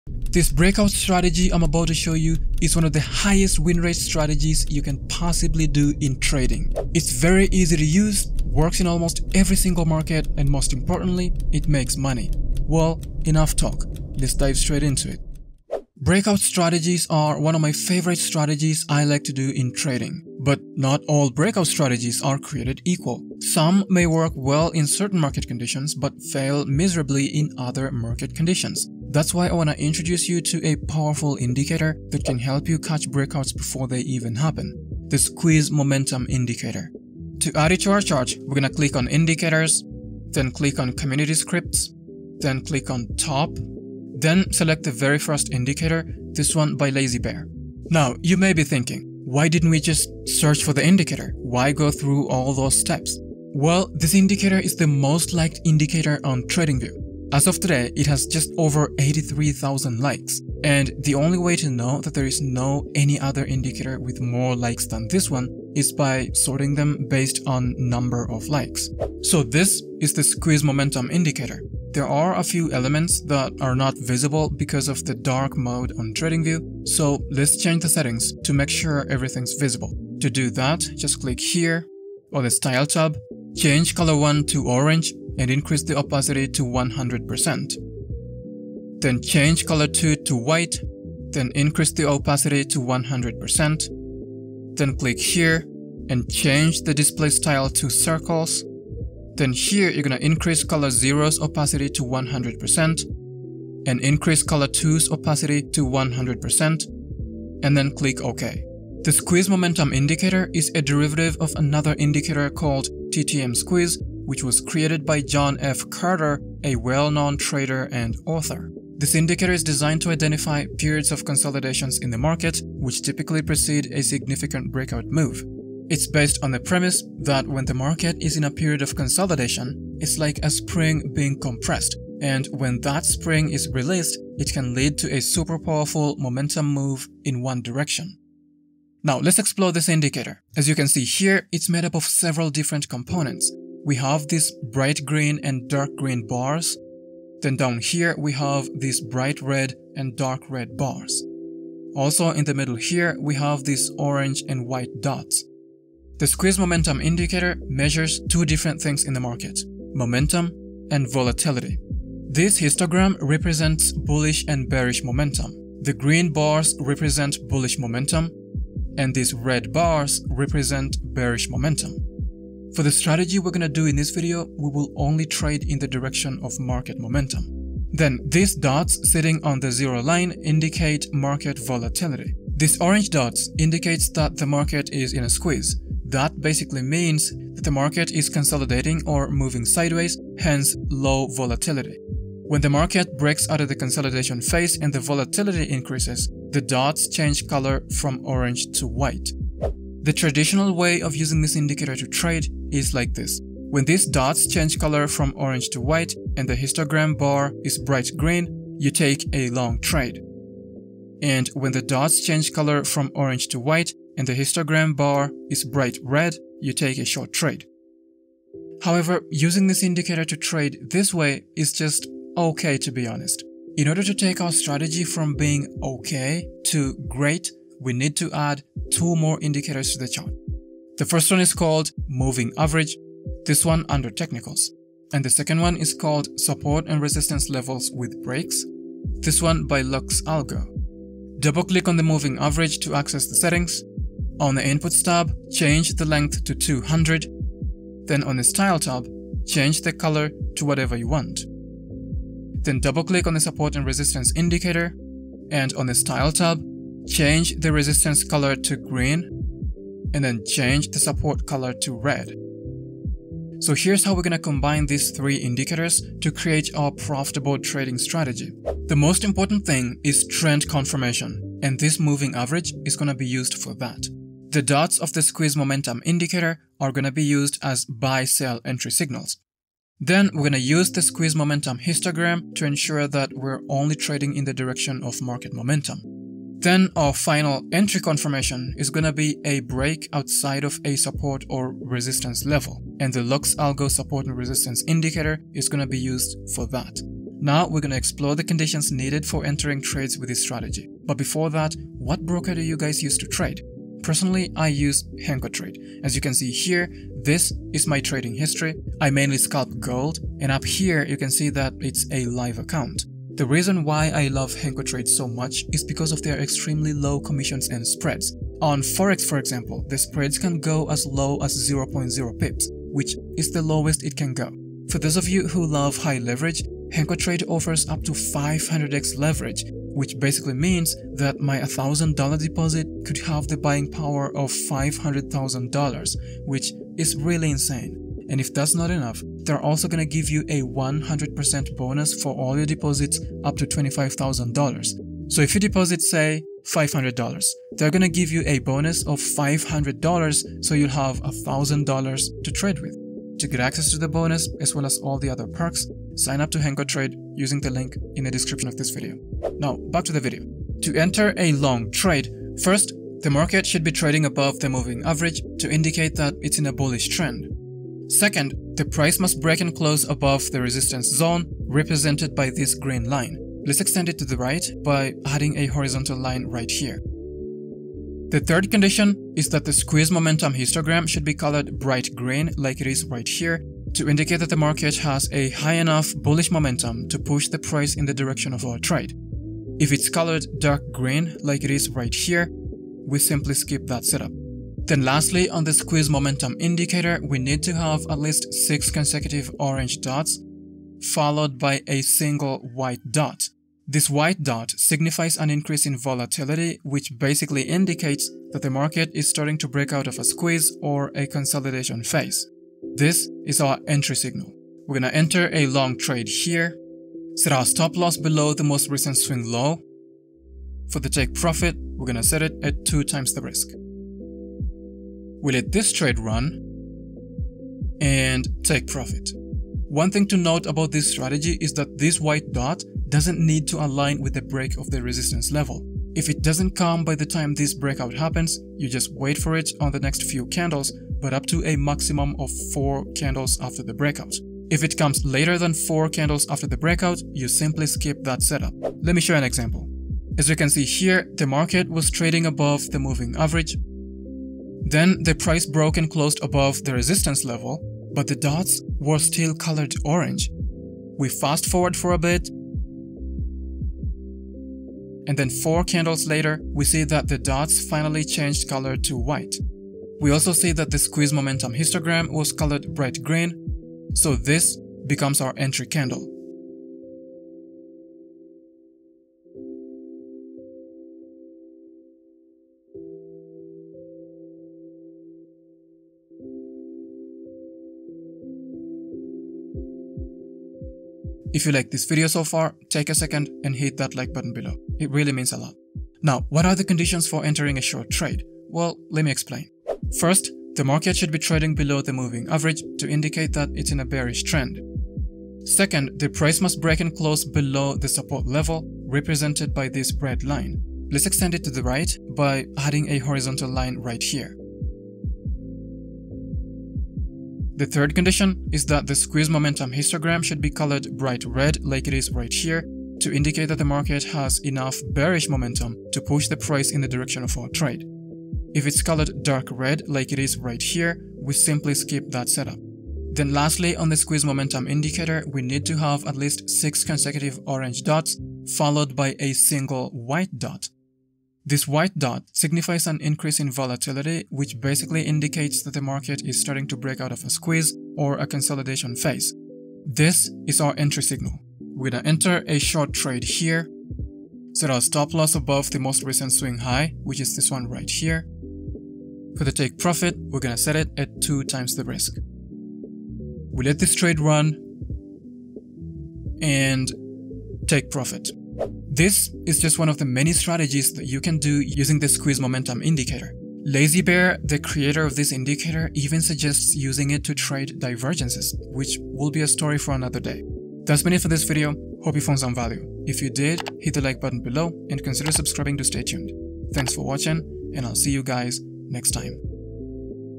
Catch Breakouts Before They Happen - Squeeze Momentum Indicator
- Are you tired of missing out on potential breakout opportunities in the market? Look no further than the Squeeze Momentum Indicator by LazyBear on Tradingview!

In this video, we'll review the Squeeze Momentum Indicator and show you how to combine it with other Tradingview indicators to create a highly profitable trading strategy. With this approach, you can catch breakouts before they happen and potentially boost your overall trading performance.

Our step-by-step guide will take you through the entire process, from setting up your Tradingview chart to executing trades based on the Squeeze Momentum Indicator and other key indicators.

✅ Lowest commission in the market!
✅ Low spreads starting from 0.0 pips!
✅ Up To 500x Leverage
✅ 100% deposit bonus, up to $25k!